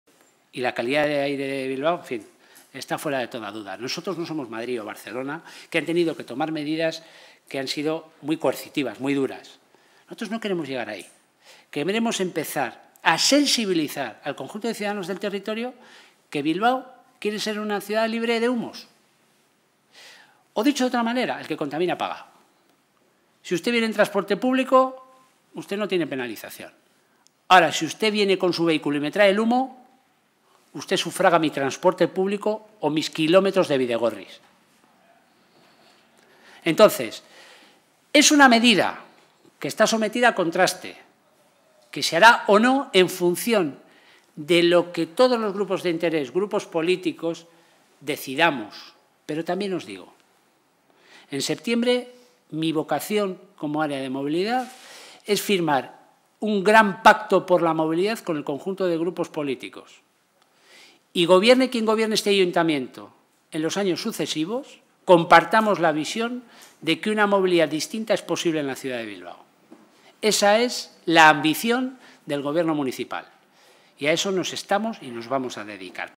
Bilbao quiere cobrar a los coches por entrar en la ciudad
El concejal de Movilidad del Ayuntamiento de Bilbao, Alfonso Gil, ha anunciado que el equipo de gobierno decidirá en septiembre si comienza a aplicar un peaje disuasorio a los 20.000 vehículos privados que se calcula acceden a la ciudad entre las siete y las nueve de la mañana, el 80% de ellos, con un único ocupante.